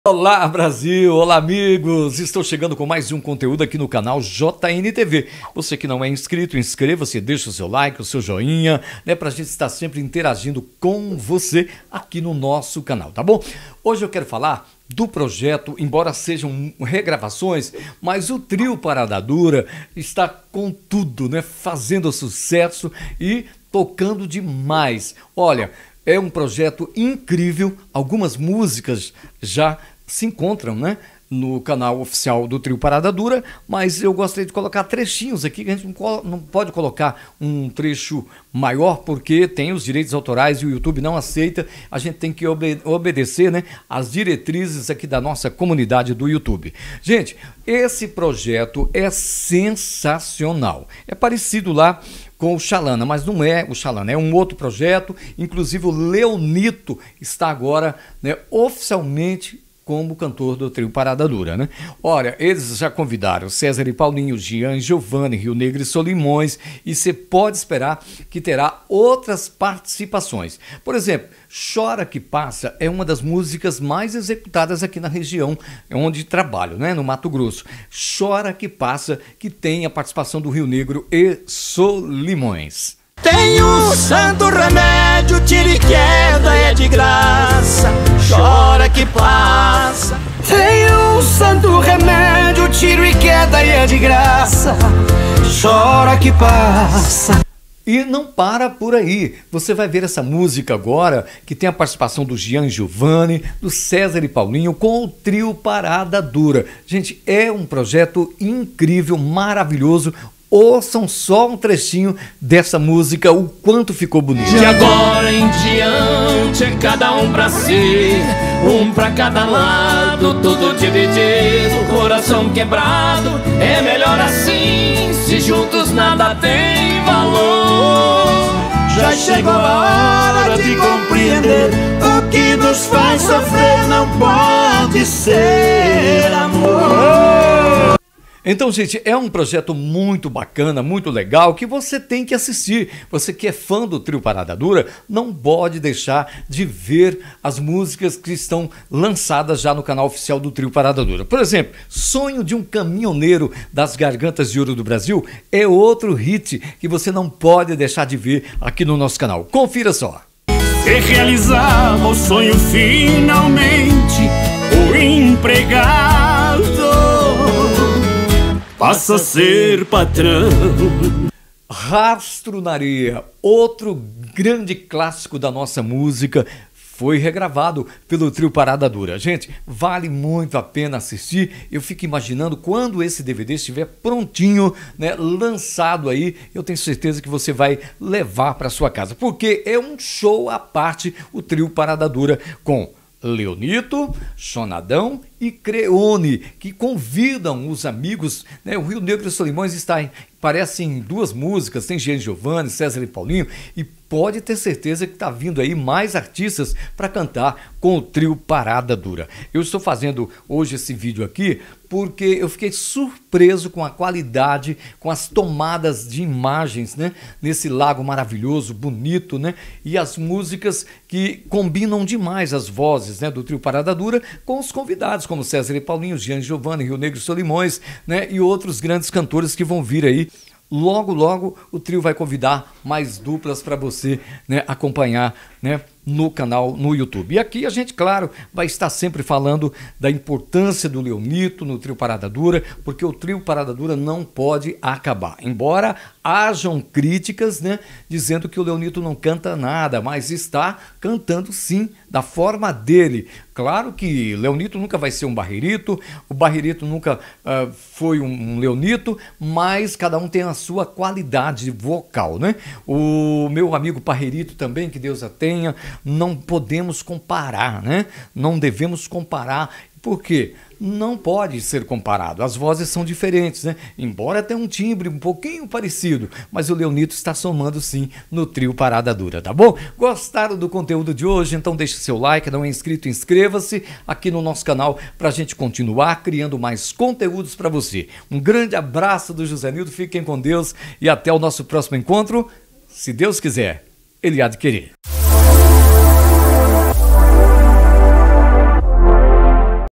O TRIO PARADA DURA ESTÁ CADA VEZ MAIS VIVO. VEJA QUE LINDO PROJETO GRAVADO NO LAGO FURNAS.
Neste vídeo o jornalista José Nildo fala do novo projeto do Trio Parada Dura no Lago de Furnas. Como não se apaixonar com o Trio Parada Dura gravado em Furnas. Veja no JNTV o comentário do Jornalista José Nildo sobre o Trio Parada Dura. O Trio Parada Dura conta com a voz marcante do cantor Leonito. Descubra neste vídeo como está o projeto do  Trio Parada Dura gravado no Lago de furnas. Assista este vídeo e veja como está a nova formação do Trio Parada Dura. Trio Parada Dura.